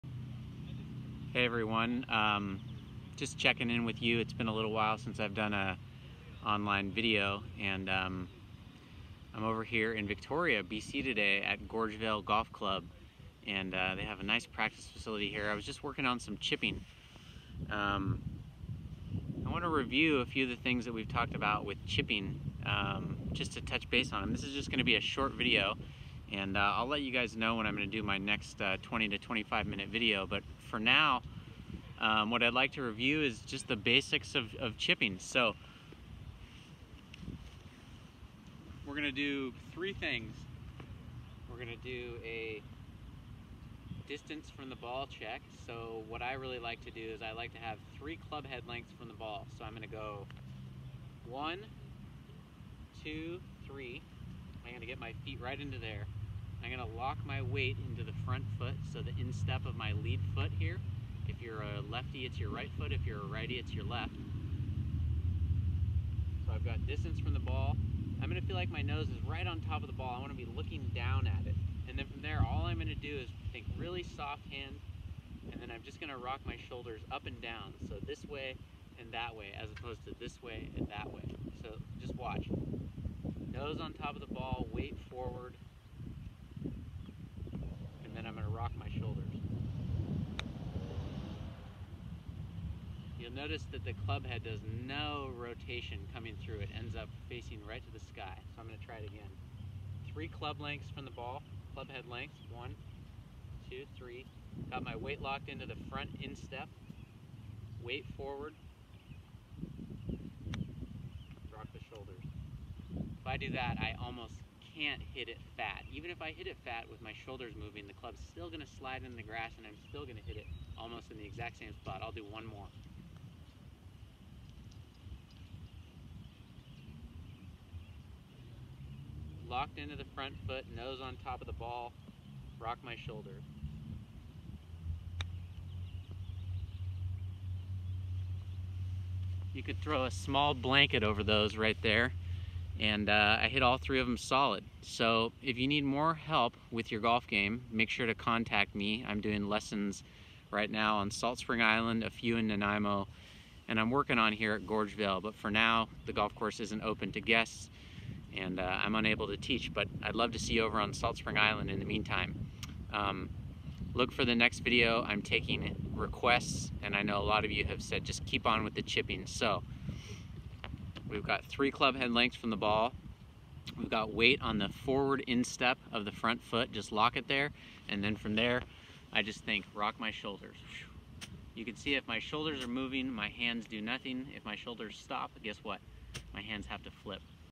Fundamentals of Chipping with Isaac Weintraub International Golf
Here in this video, we re-visit the fundamentals of chipping. Chipping 101, a short video from Gorge Vale Golf Club on a simple technique for solid contact. Isaac Weintraub Golf, Chipping. Salt Spring Island Golf Club.